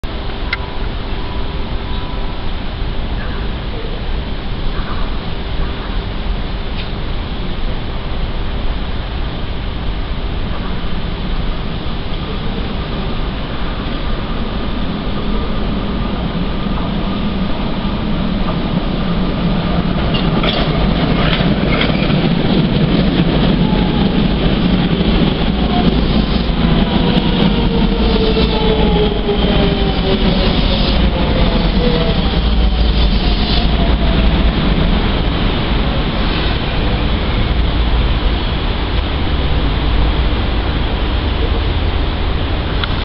地下鉄東山線５０５０形到着。
２０１５年８月撮影。
上社にて。